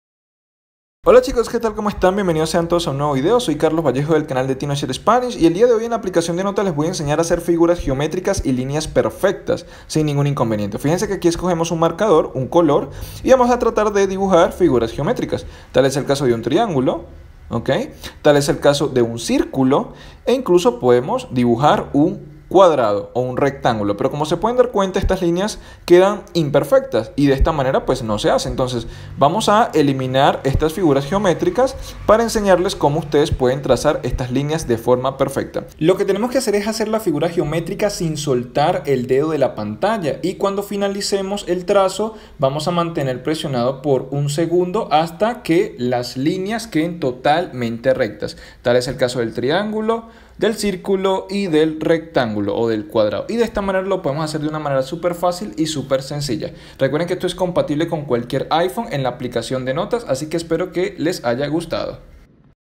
Cómo dibujar formas perfectas en iPhone/iPad
Vamos a ver cómo dibujar formas perfectas en iPhone.